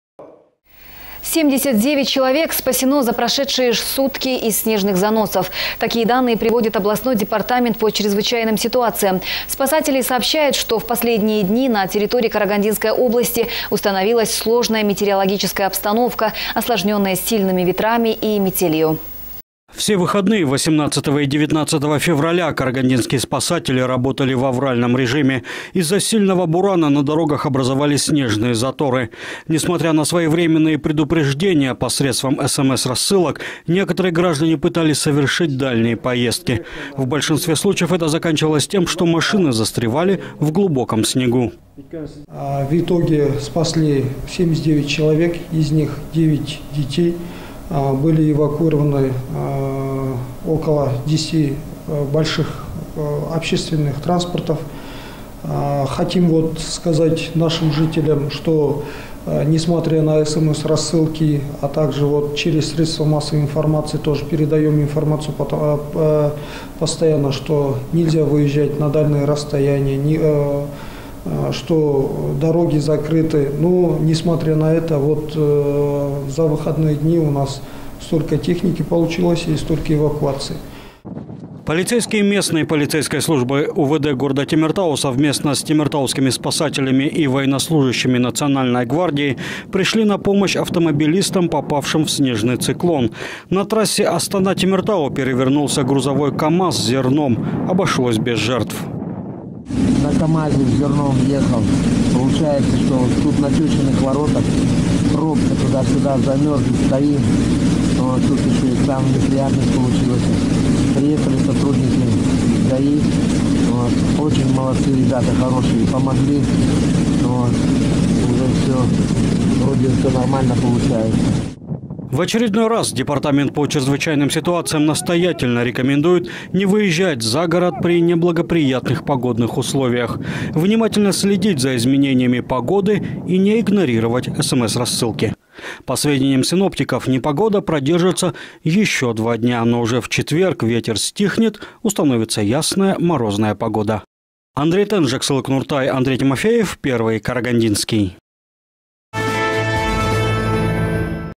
Все выходные 18-го и 19-го февраля карагандинские спасатели работали в авральном режиме.
Из-за сильного бурана на дорогах образовались снежные заторы. Не смотря на своевременные предупреждения посредством СМС-рассылок, некоторые граждане пытались совершить дальние поездки. В большинстве случаев это заканчивалось тем, что машины застревали в глубоком снегу.

Полицейские Местной полицейской службы УВД города Темиртау совместно с сотрудниками темиртаускими спасателями и военнослужащими Национальной гвардии пришли на помощь автомобилистам, попавшим в снежный циклон. На трассе Астана-Темиртау перевернулся грузовой КамАЗ с зерном. Обошлось без жертв.

В очередной раз департамент по чрезвычайным ситуациям настоятельно рекомендует не выезжать за город при неблагоприятных погодных условиях. Внимательно следить за изменениями погоды и не игнорировать СМС-рассылки. По сведениям синоптиков, непогода продержится еще один день. Но уже в четверг ветер стихнет, установится ясная морозная погода.